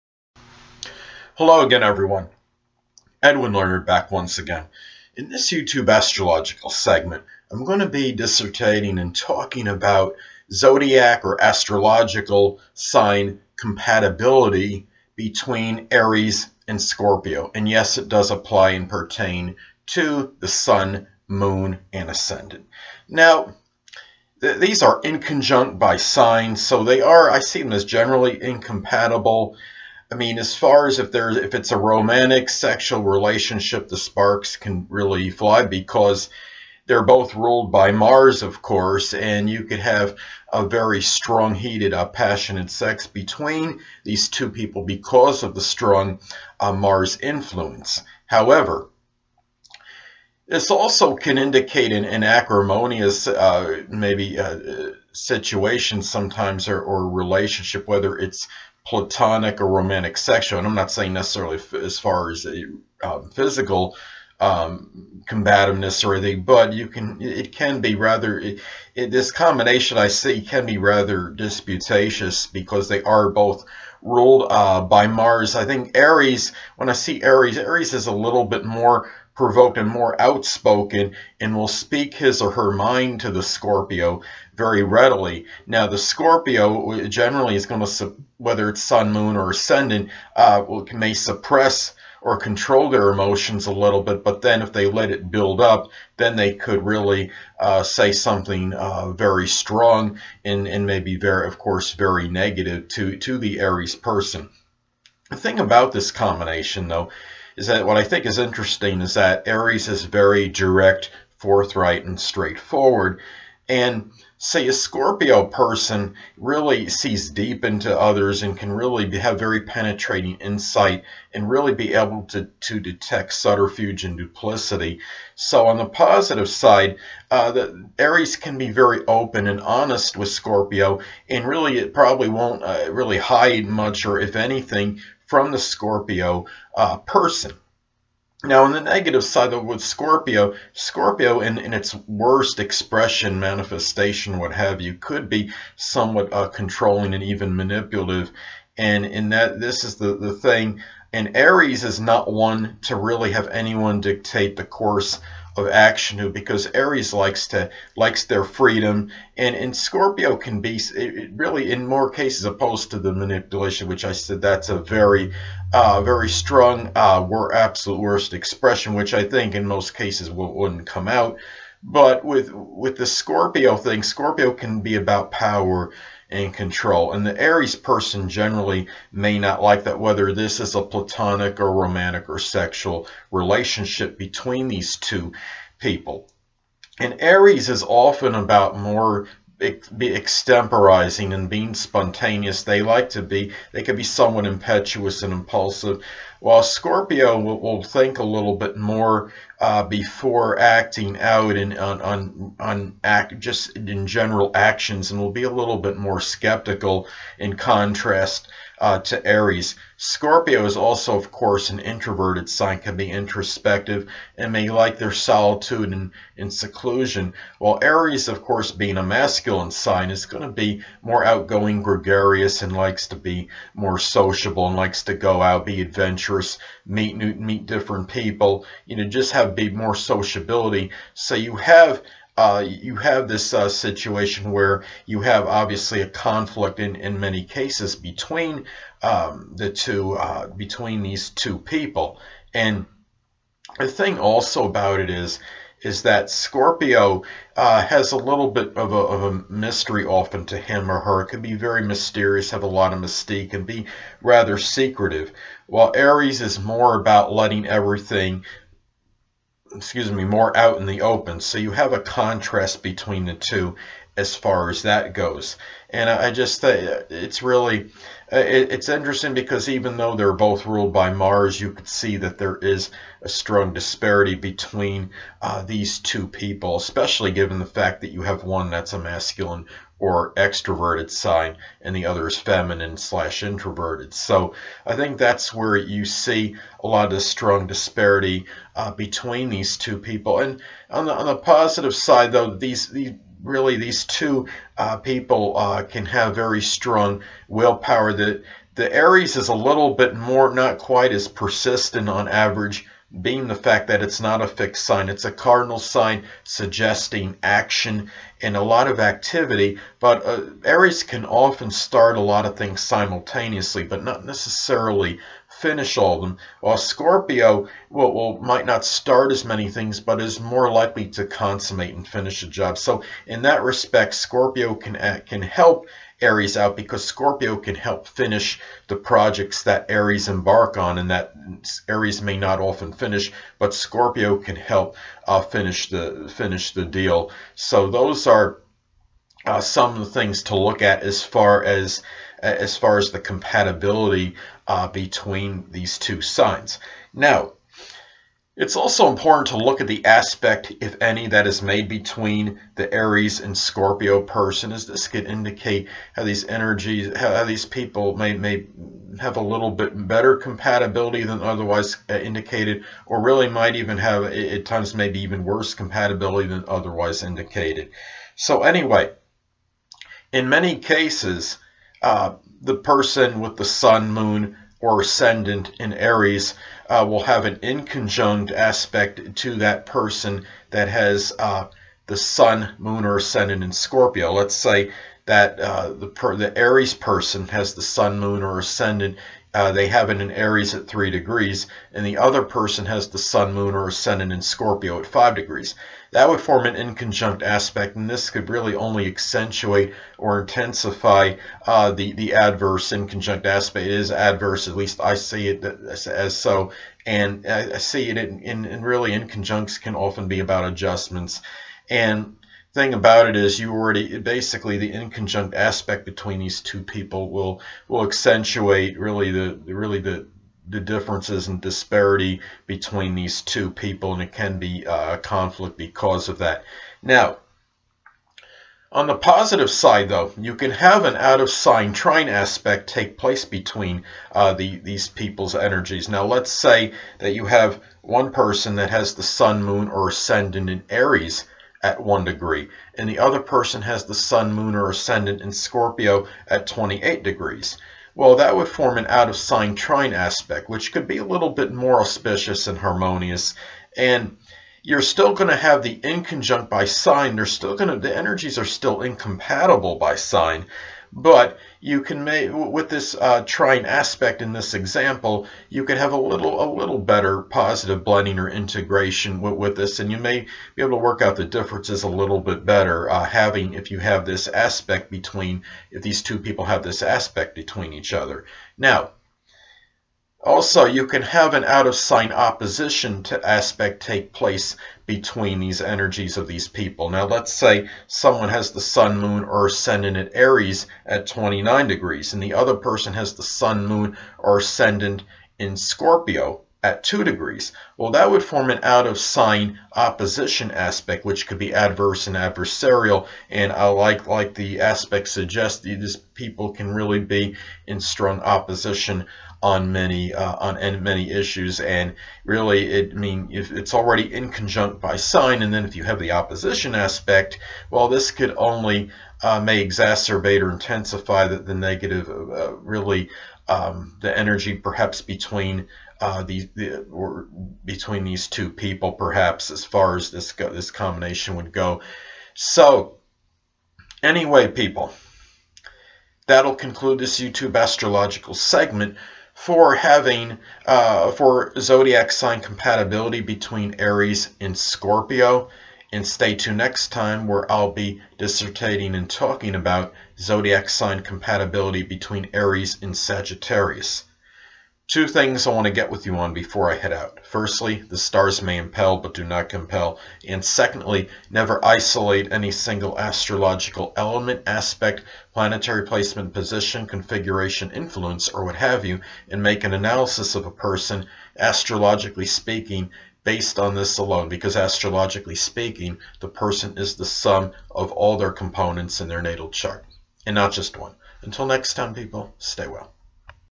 Aries/Scorpio Compatibility
Me, Edwin Learnard, talking about astrological compatibility between Aries and Scorpio (Sun, Moon, Ascendant).  Please check out my friend Vera's astrology channel as she has provocative and insightful videos pertaining to the subject. Here is the link to her channel